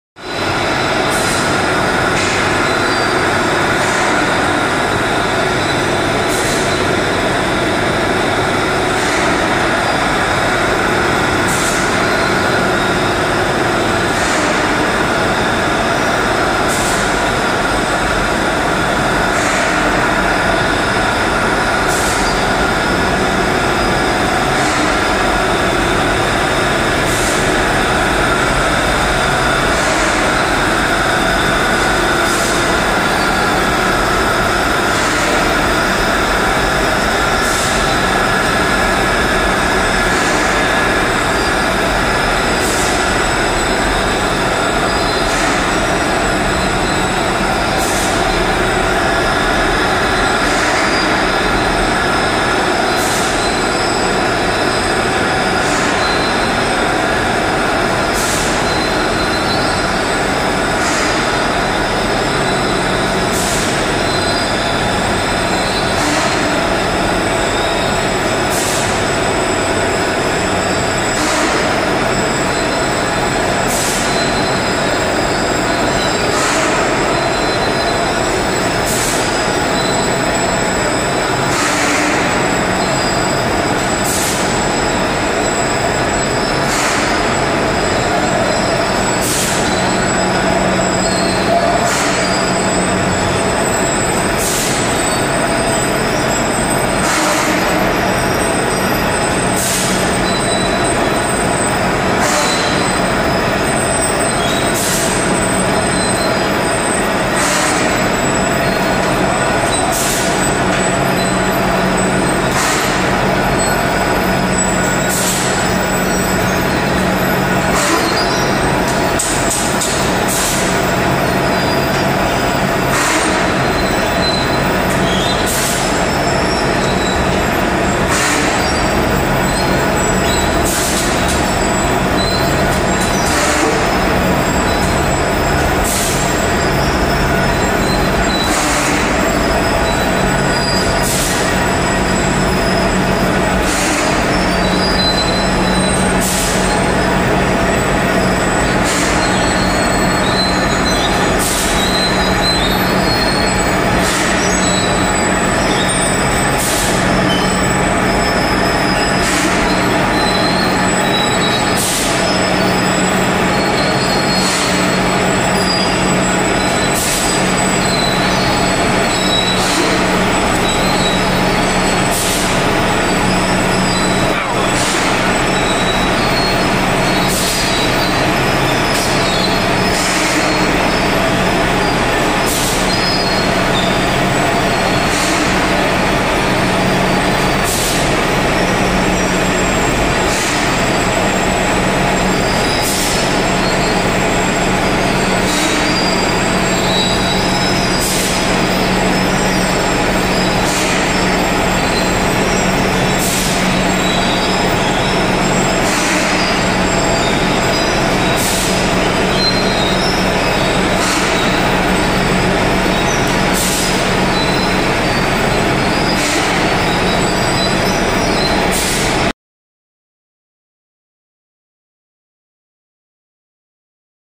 Full Process! 4000PCS/H Egg Tray Manufacturing Machine Line from Customer Feedback
Discover the inner workings of a egg tray factory as we guide you through each step of the production process. From raw materials to the final product, witness the efficiency and precision of our high-speed egg carton machine in action.

0:00 factory tour for egg tray manufacturing
0:30 multi-layer egg tray dryer
1:30 egg tray manufacturing machine production process
2:05 great feedback from customers
3:00 egg tray packing process

This egg tray manufacturing machine can produce 4000  to 5000 top-quality egg trays or egg cartons per hour. It has other application, including producing apple trays, or other fruit trays, cup carrier, seedling trays, shoe trays, etc.